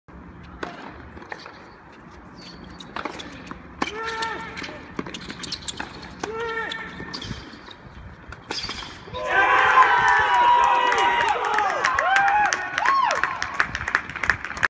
Joe Di Guilio wins over Georgia in Pac-12/SEC Showdown
Match point for Joe Di Guilio's win over Paul Oosterbaan at #4 singles. UCLA went up 3-2 against Georgia after the win at the Alfred A. Ring Tennis Complex on February 4, 2017 in the Pac-12/SEC Showdown. Final Score: UCLA def. Georgia 4-3